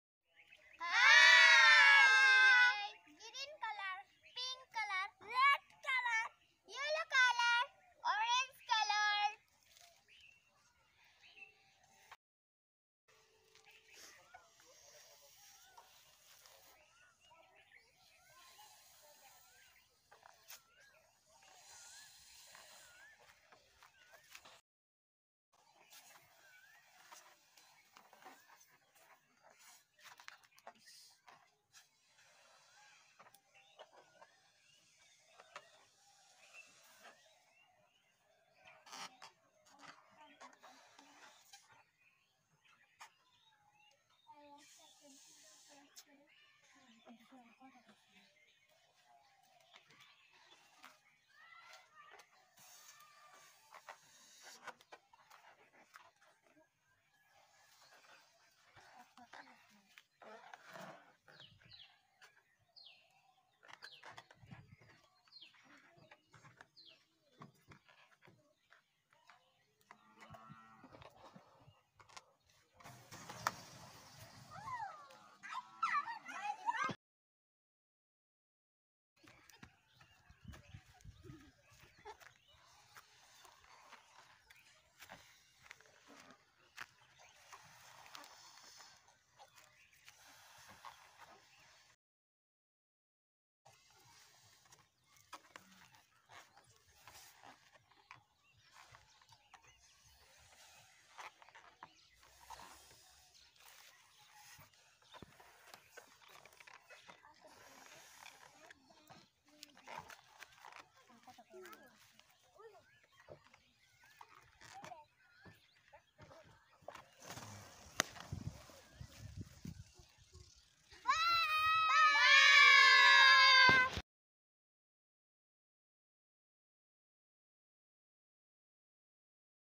Outdoor fun with rocket balloon and Learn colour for kids episode-9
outdoor fun with colourful balloon and learn colour for kids balloon show episode-9